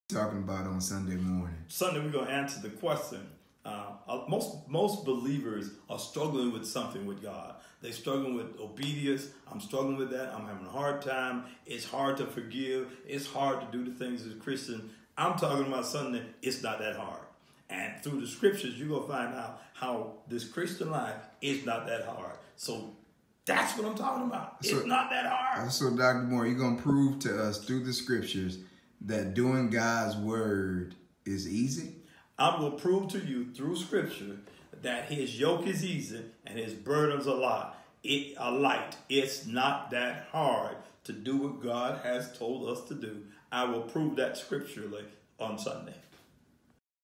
It's Not That Hard
The commandments of God are not hard. This sermon is tailored to teach that you can obey God's commandments because of His strength, His strategy, and His source. 1 John 5:1-4.